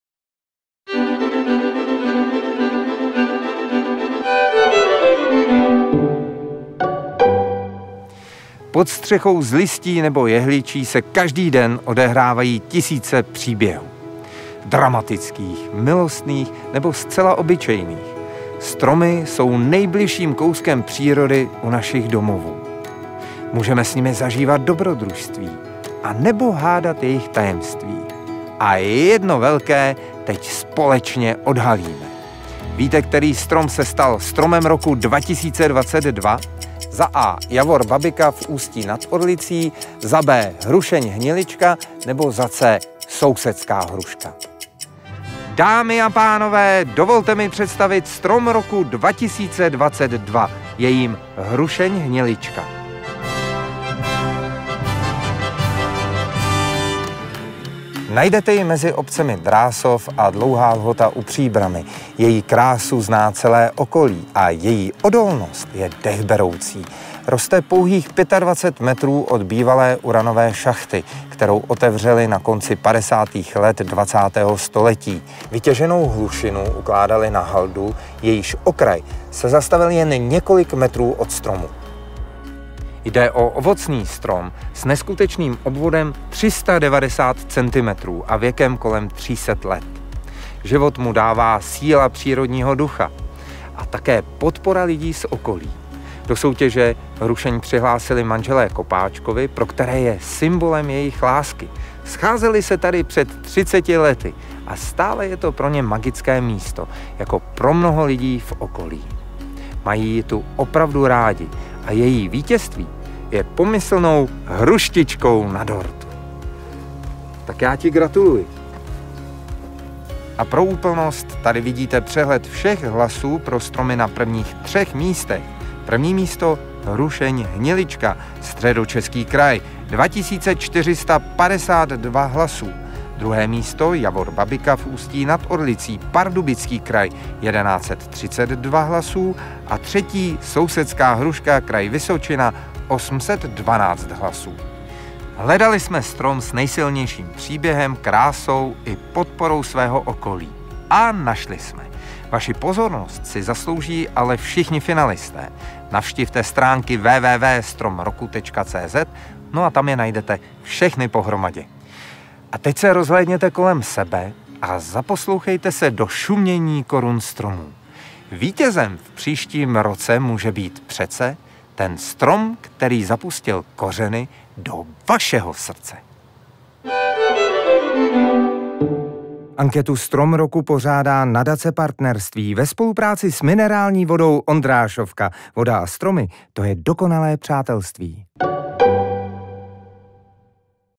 Vítězka ankety Strom roku 2022 – Hrušeň hnilička
Vylosovala si soutěžní číslo 1. Vedla průběžné pořadí a prvenství si udržela až do samotného konce tříměsíčního klání. Vytrvalost je jí totiž vlastní. Hrušeň hnilička rostoucí mezi obcemi Drásov a Dlouhá Lhota u Příbrami získala 2 452 hlasů a stala se Stromem roku 2022.